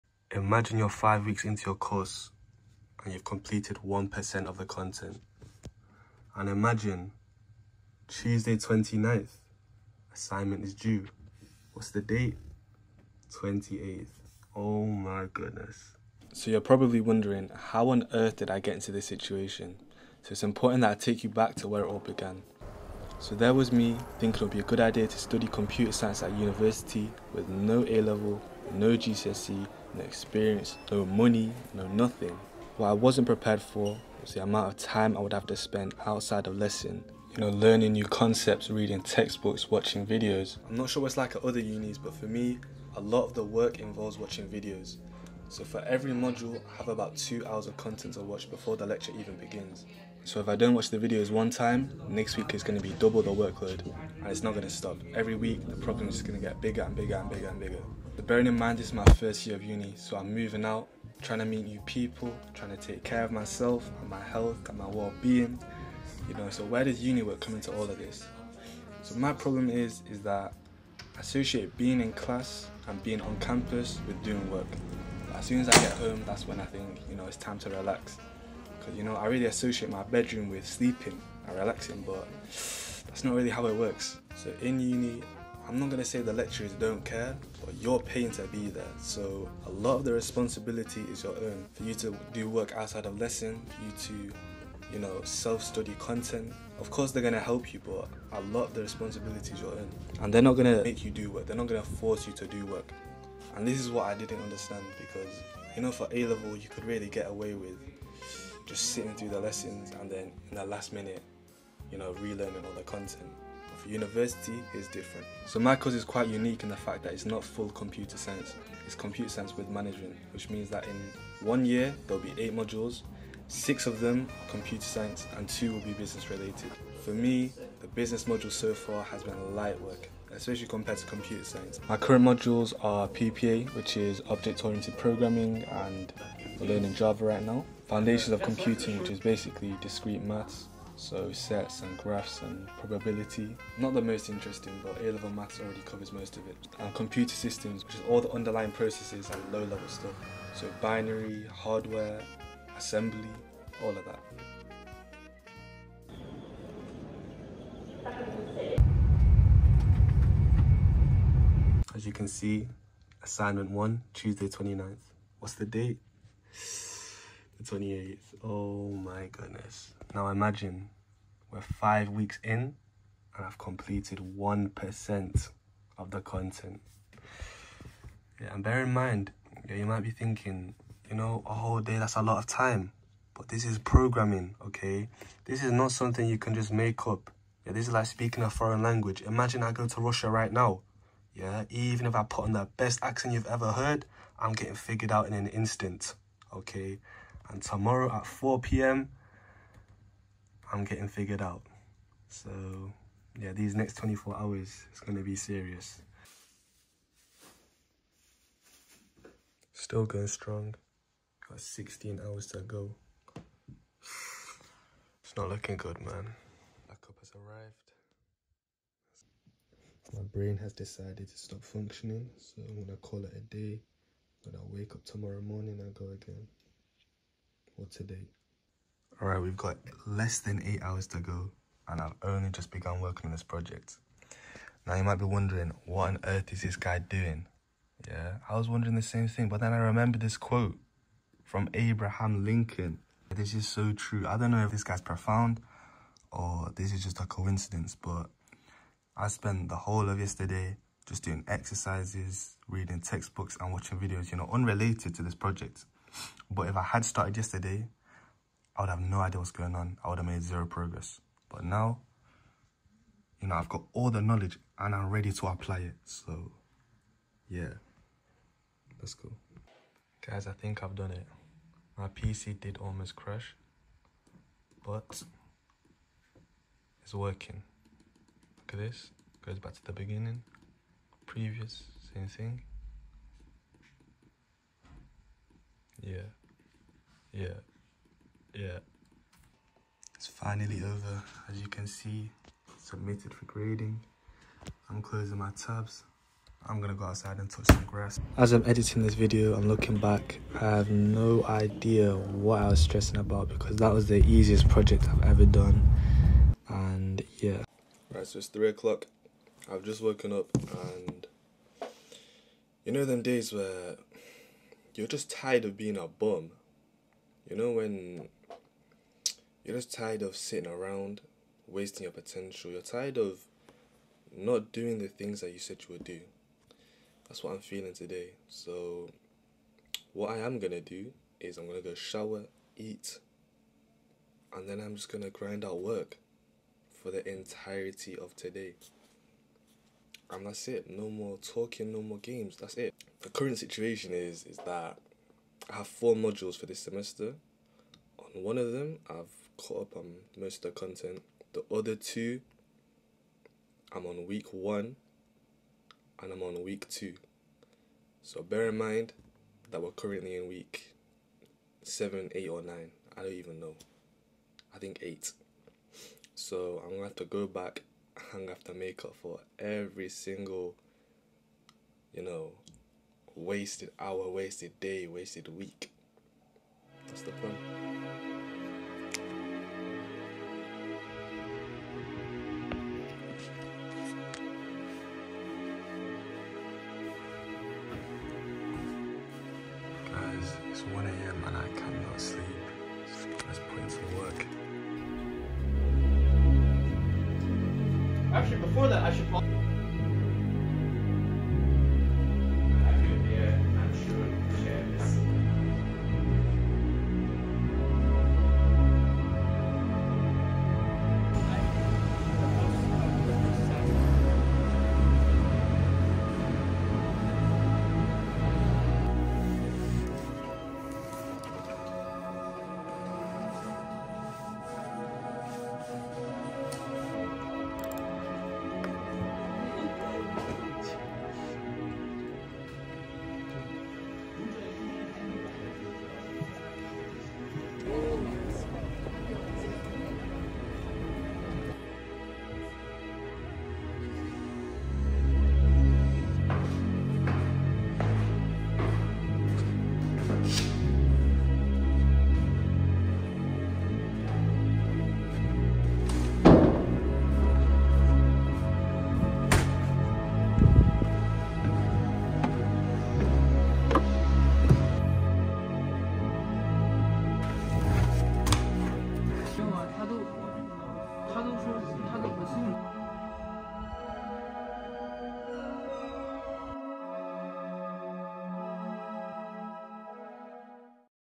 Starting Computer Science at University (from Scratch, First Year)
Recap of what it was like to start a Computer Science degree at a London university, with no previous experience. Modules, coursework, trying to balance a life outside of studying, falling behind and catching up on missed content, etc.

Somehow managed to complete my first coursework and submit it on time, but by the time this videos released, the second one will already be set so make sure to follow along for the next one and to see the progression.